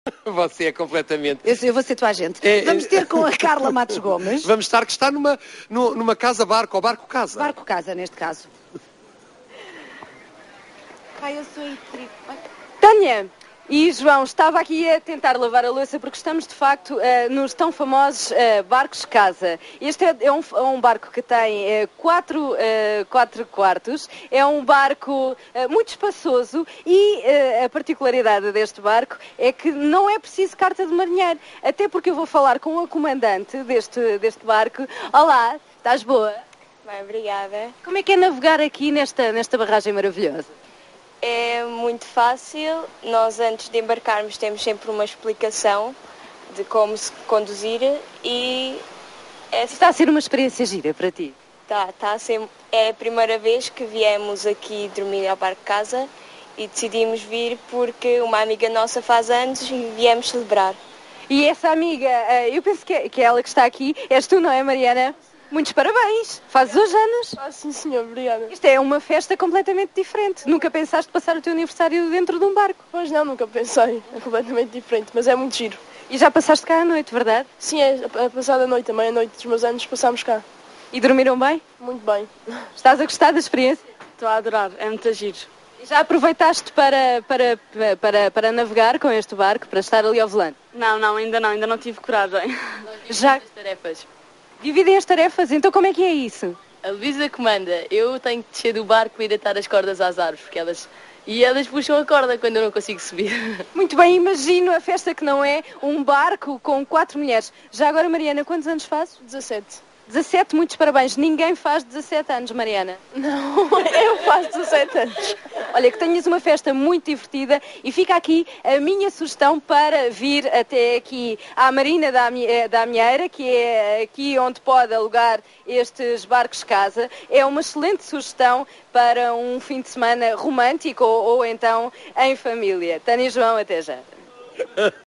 RTP2010-08-07-entrevista_bc_luisa_amigas.flv
Excerto de Programa em Directo emitido pela RTP1 em 8/7/2010.
Entrevista a Luisa Leal da Costa e amigas a bordo de um Barco Casa